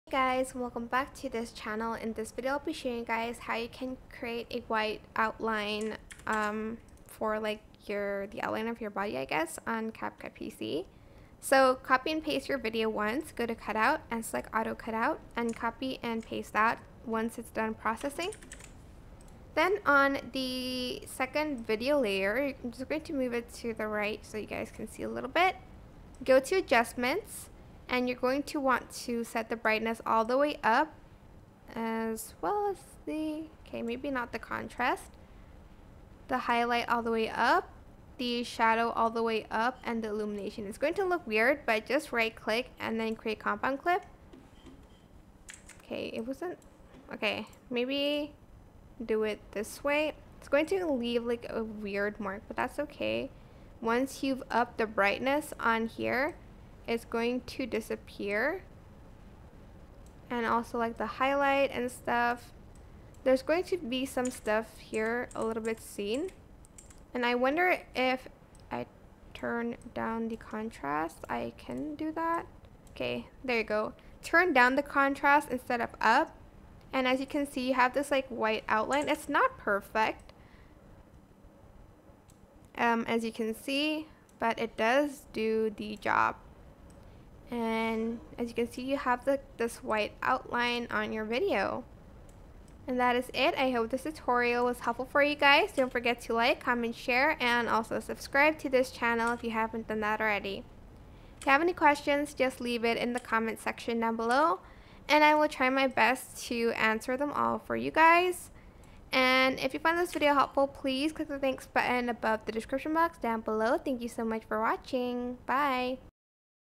Want to Create a White Outline Effect on Capcut PC? Learn How to Do It! Step-by-Step Tutorial
Want to create this white border around your body in CapCut PC? Here's how you can do that.